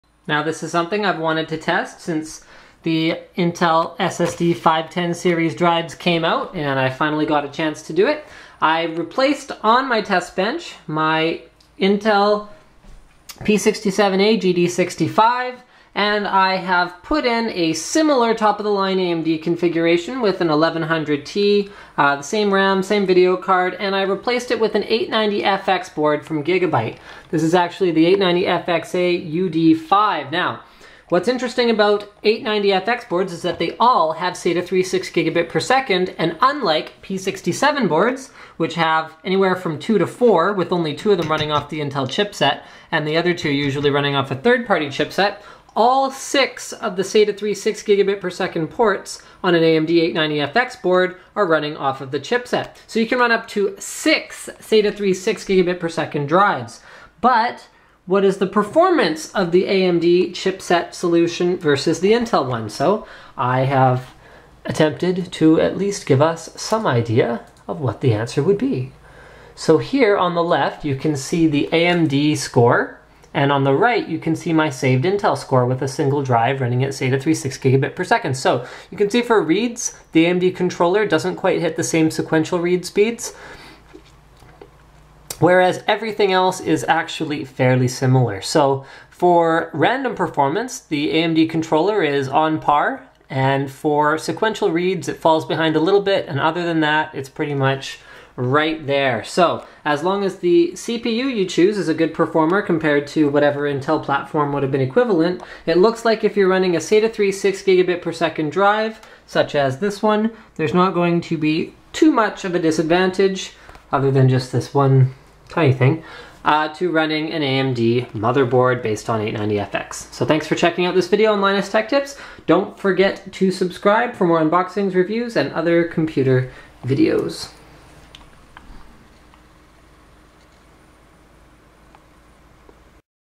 Intel P67 SATA3 6Gbps Controller vs AMD 890FX Controller Linus Tech Tips
Find out which one performs better with the new Intel SSD 510 series drive!